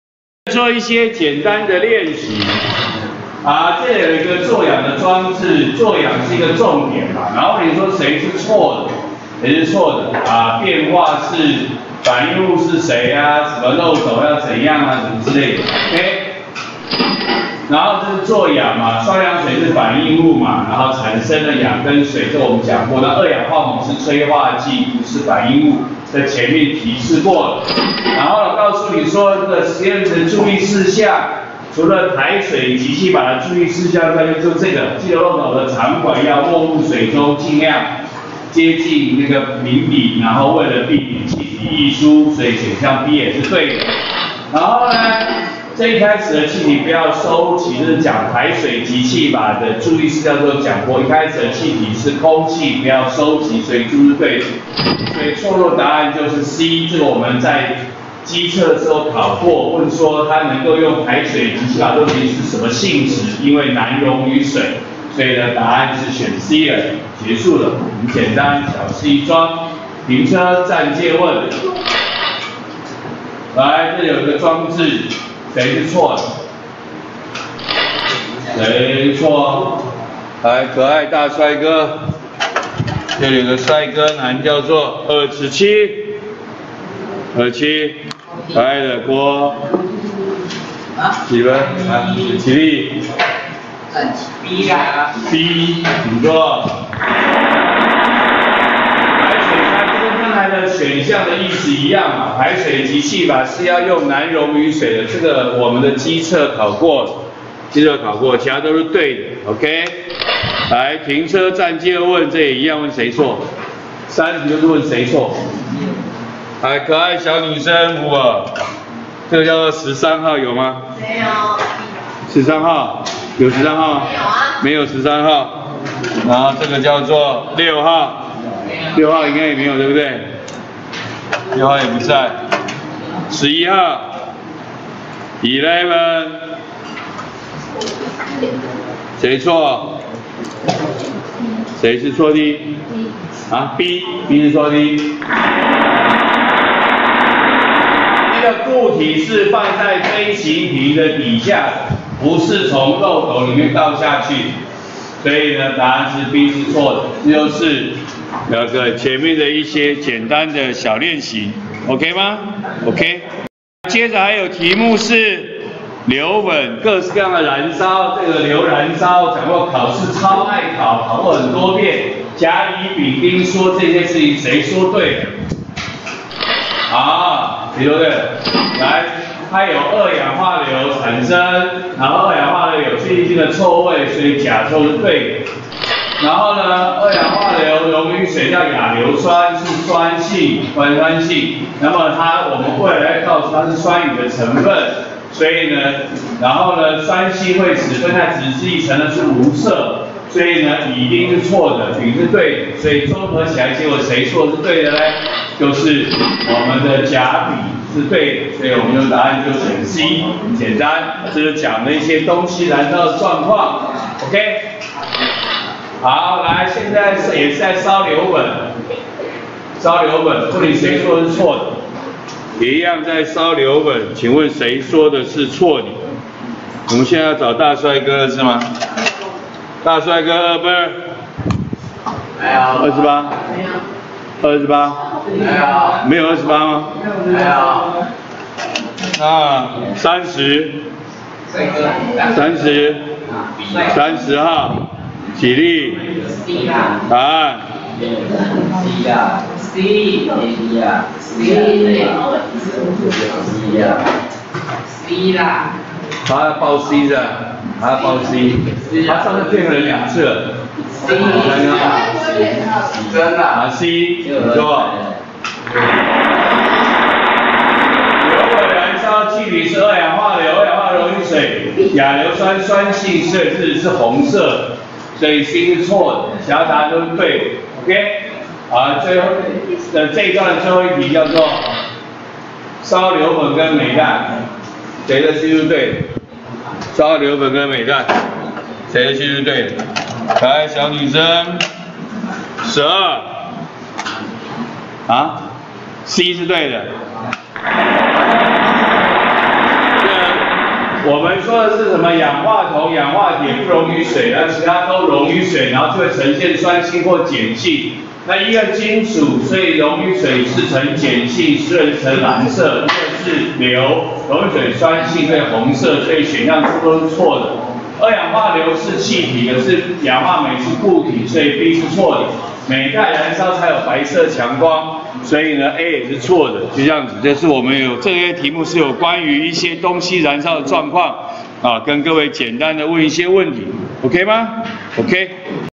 106學年複習空氣09：例題03、04；練習05～08
例題03、練習05、練習06問關於雙氧水製氧，敘述何者錯誤。例題04問關於硫燃燒，敘述何者正確。練習07則是問何者錯誤。練習08問關於鎂及硫燃燒，敘述何者正確。